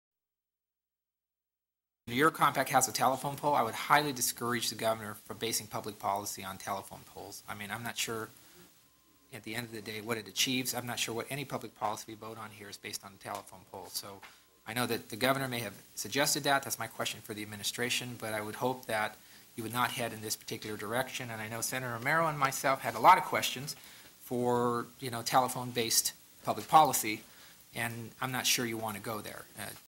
No way to run a state!
Approve casino expansions through phone polls?  Too crazy to believe.  Alas, Senator Florez voted "Aye" anyway!